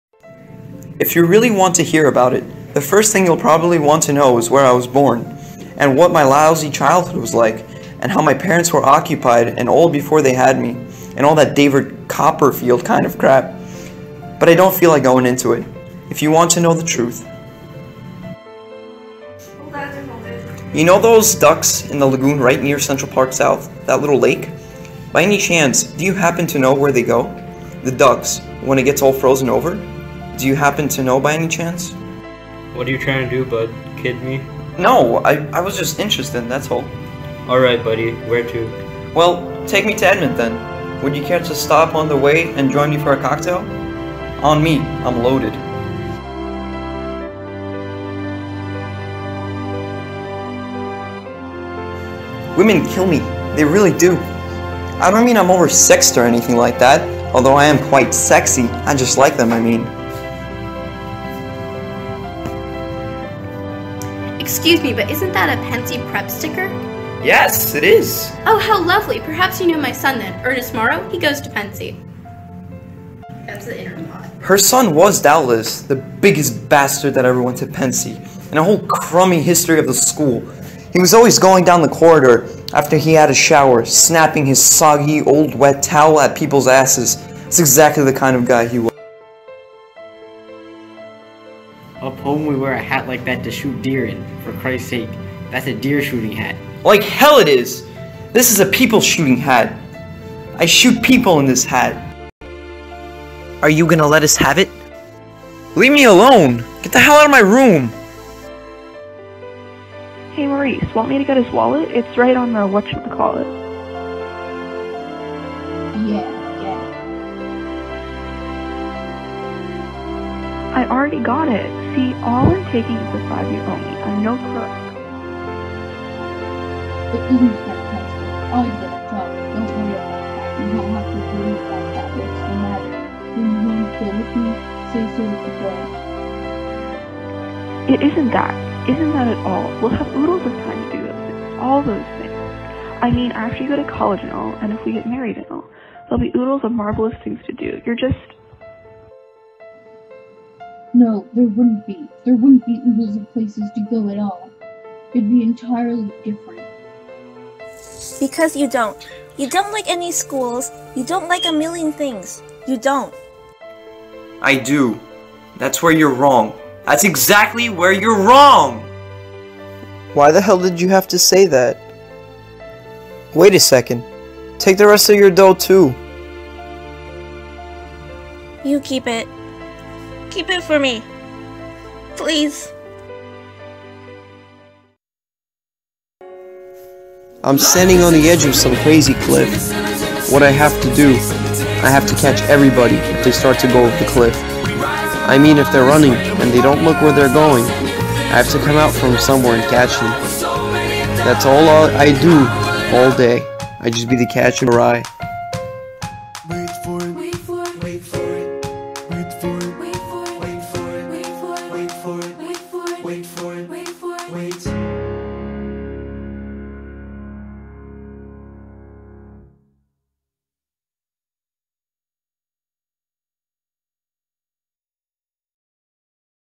ENGLISH PROJECT TRAILER: Catcher in the Rye 🌟
😊 So in my school,  we got an english project to make a trailer for our assigned books in our groups.  My book was "Catcher in the Rye" and my group was like "AYE LETS GET IT ANIMATED INSTEAD OF FILMING US ACT CAUSE YEA"
Me: "YEEEEEH"

(By the way I had like 2 days to animate this,  I know its trashyyyyy)

Every groups supposed to post it on youtube so bam here it is 😂

😍💖💖💖 BUT CAN I JUST SAY,  MY GROUP IS ADORABLE AND FUN AND IM LUCKY TO GET THEM,  THEY SO COOL 👌

📌 Credits at end of video,  but I'll just say them here

Some scenes; voice of Sally and Sunny: Aoife H. (👍 Super chill,  I can't even)

Some scenes,  basic storyboard; voice of the mother: Aviva S.  (💯She got them leadership skills)

Some scenes, help with background; voice of Cab driver: Rayhan Z.  (🐢 He's so shy,  aweee)

Some scenes; voice of Ackley: Andrew T. (✌ Secretly savage)

Some scenes, moral support; voice of Holden and Maurice: Aleksei A.  (👉😎👉DANKEST BOIIIII)

K BAI~

If you have anything you’d like to send me, you can:
- Send it to me on discord at scarlettispaghetti